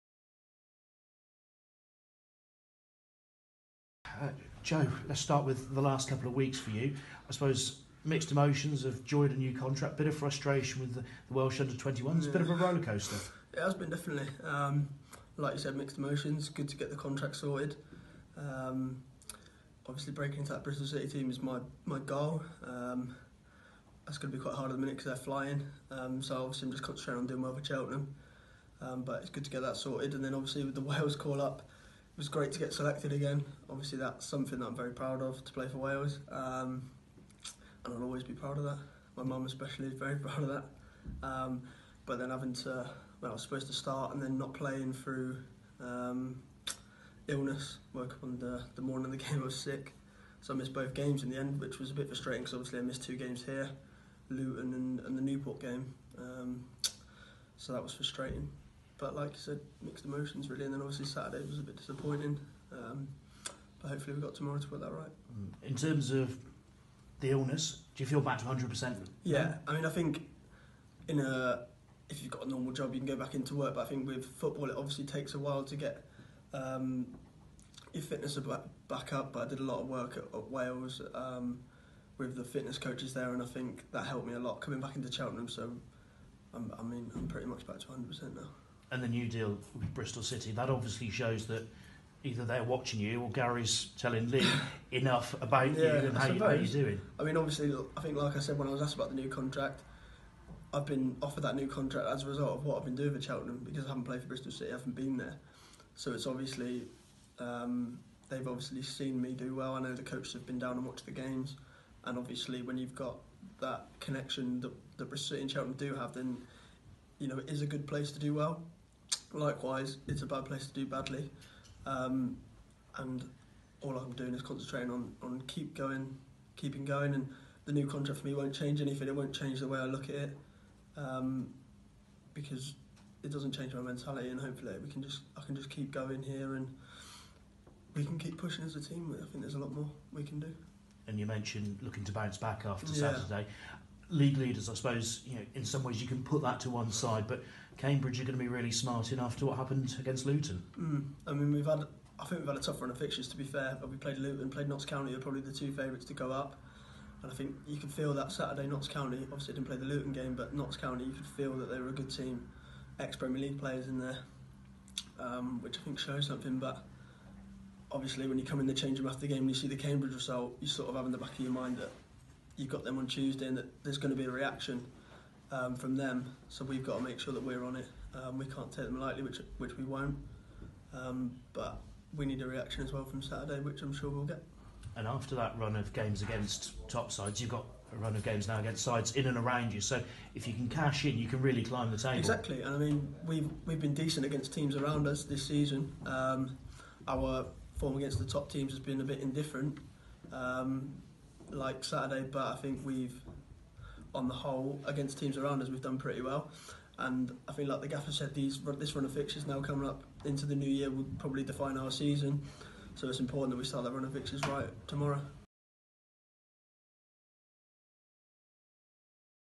Robins TV: Joe Morrell previews Cambridge United clash
Midfielder Joe Morrell looks ahead to the Sky Bet League Two clash with Cambridge.